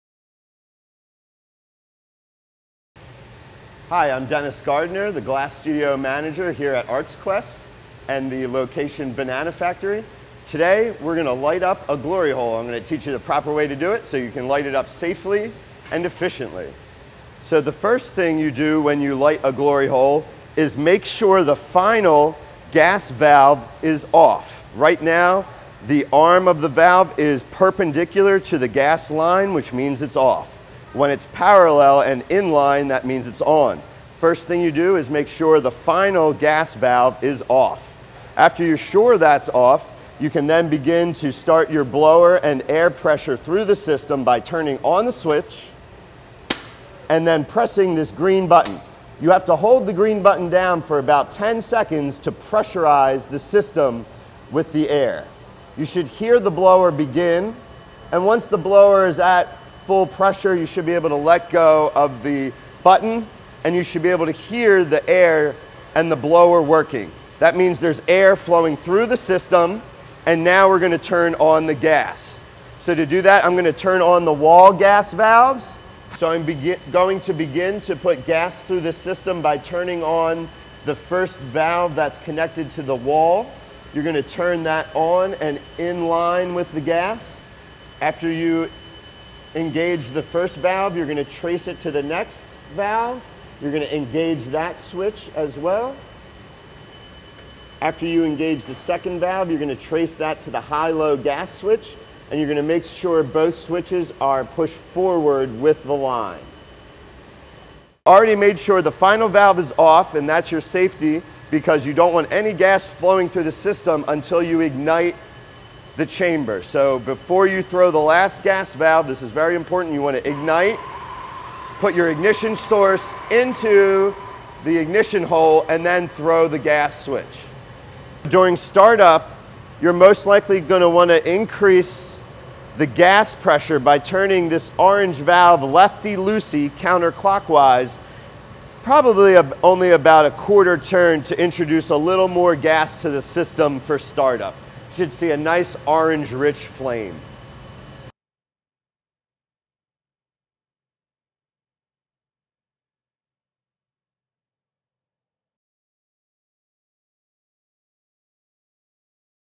ArtsQuest Glass Studio: Re-Heating Chamber Fire Up Tutorial
Dennis Gardner from the ArtsQuest Glass Studio in Bethlehem, PA shows you how to re-heat the chamber fire up.

Take an upcoming glass with the ArtsQuest Glass Studio at The Banana Factory Arts Center